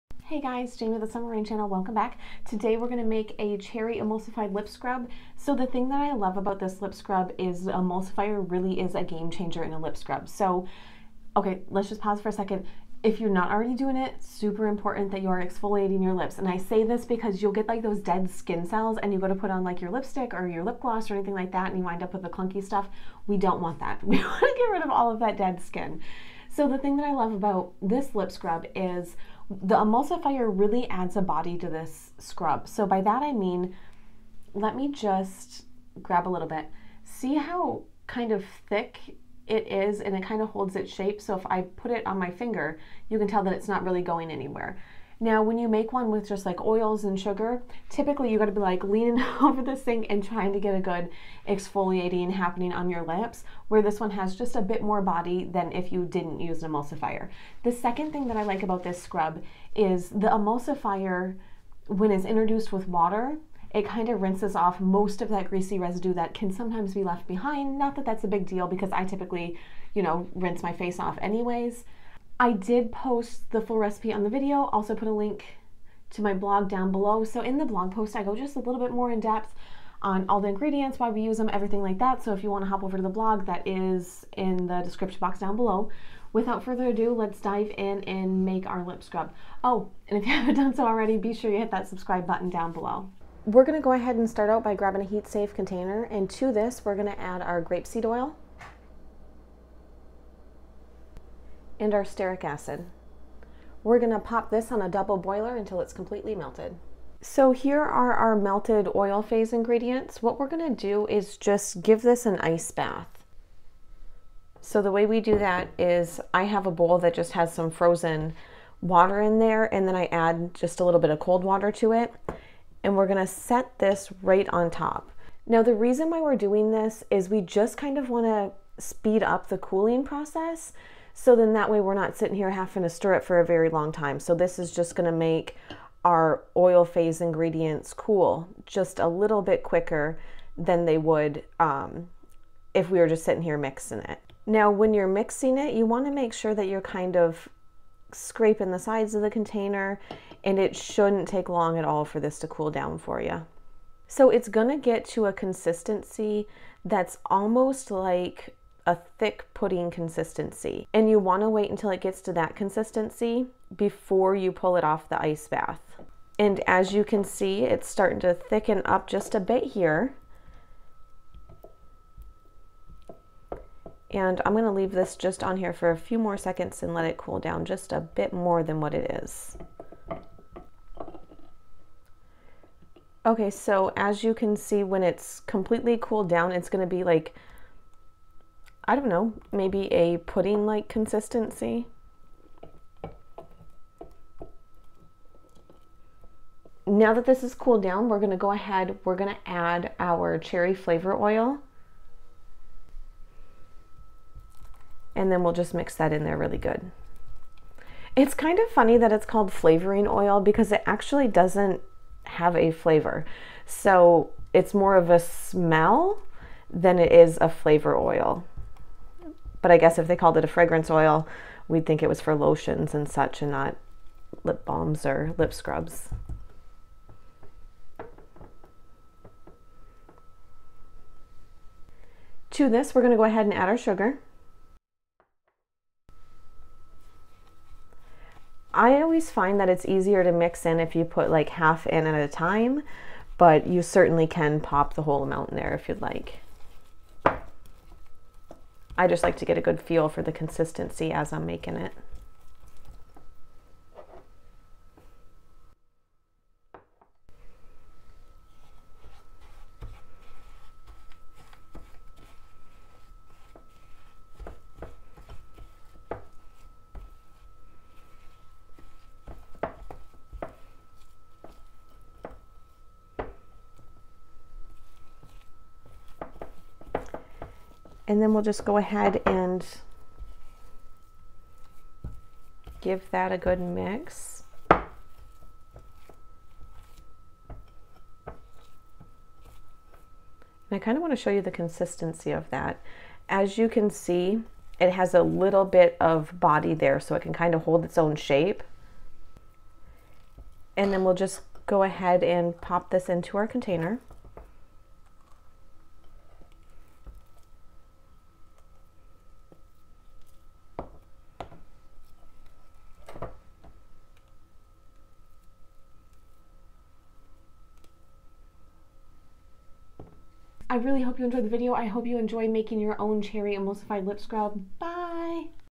How to make Cherry Emulsified Sugar Scrub (includes recipe)
I love how this cherry emulsified lip scrub rinses right off without leaving behind a greasy residue. This scrub is gentle enough to use on your lips but powerful enough to slough off the dead skin cells, leaving your lips soft as silk. With only 4 ingredients, you can whip this simple recipe up in just a few minutes.

• 0.33 ounce | 11% Stearic Acid
• 0.78 ounce | 26% Grapeseed Oil
• 1.86 ounce | 62% Granulated Sugar

Heat safe bowls
Pouring Spout Container
Temperature Gun
Immersion blender

0:00 intro
1:34 oil phase
3:18 cool down phase